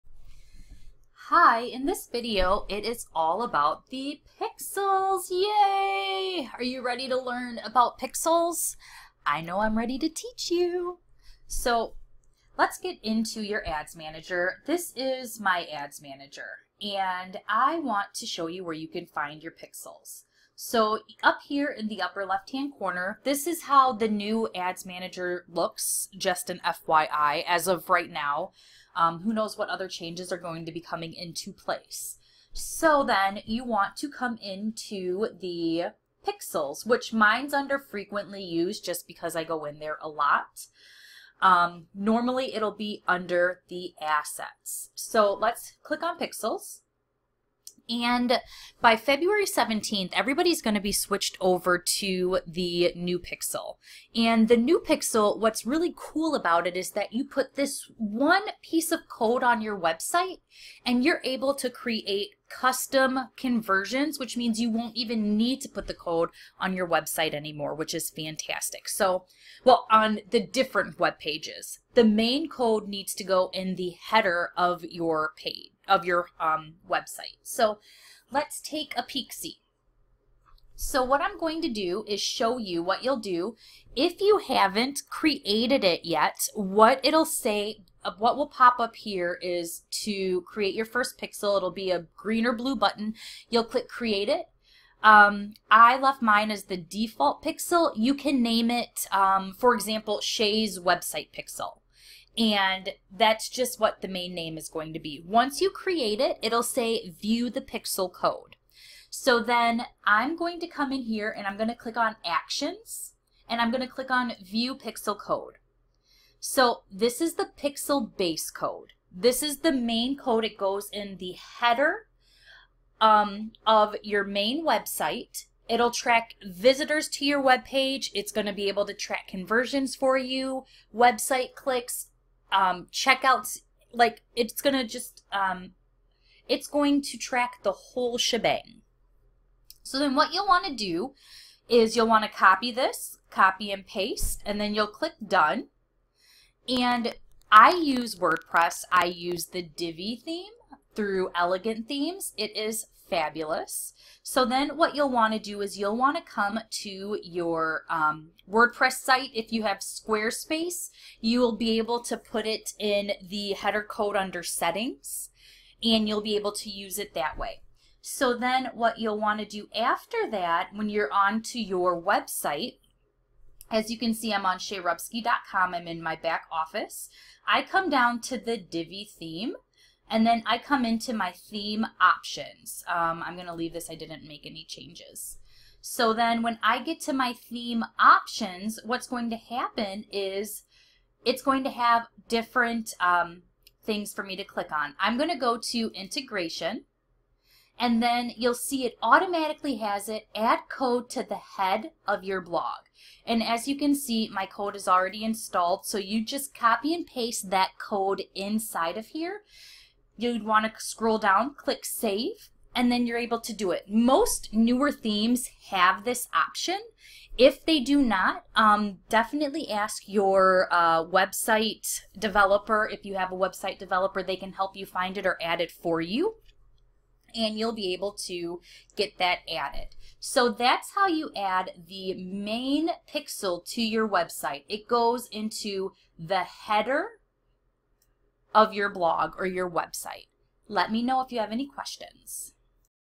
How To Add The New Facebook Base Pixel To Your Website
In this video, I show you how I add it to my Wordpress website through my wordpress theme and how to set it up if you haven't set up the new Facebook Base Pixel yet.